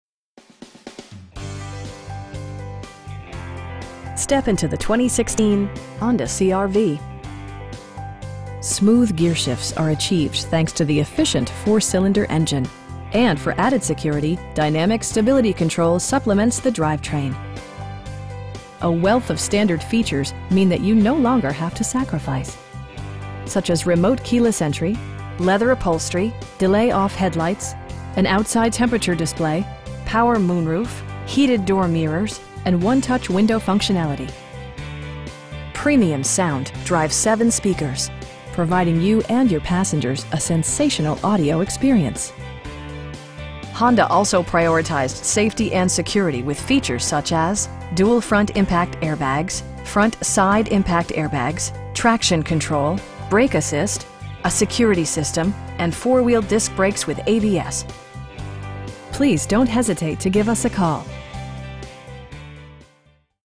2016 Honda CR-V EX-L FWD in Orlando, FL 32837
11051 S Orange Blossom Trail

Step into the 2016 Honda CR-V. Smooth gearshifts are achieved thanks to the efficient 4 cylinder engine, and for added security, dynamic Stability Control supplements the drivetrain. A wealth of standard features means that you no longer have to sacrifice. Such as remote keyless entry, leather upholstery, delay-off headlights, an outside temperature display, telescoping steering wheel, power moon roof, heated door mirrors, and 1-touch window functionality. Premium sound drives 7 speakers, providing you and your passengers a sensational audio experience. Honda also prioritized safety and security with features such as: dual front impact airbags, front SIDE impact airbags, traction control, brake assist, a security system, and 4 wheel disc brakes with ABS. You will have a pleasant shopping experience that is fun, informative, and never high pressured. Please don't hesitate to give us a call.